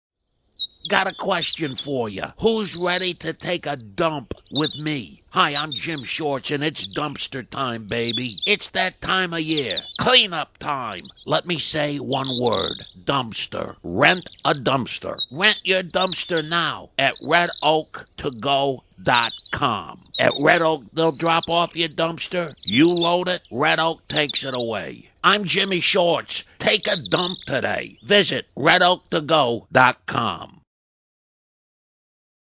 Jimmy recommends taking a dump?
Are you ready for Jim‘s unique way of explaining how Red Oak Disposal Services dumpster can help you finish any project at your home business or community?
 No birds were injured in the making of this animation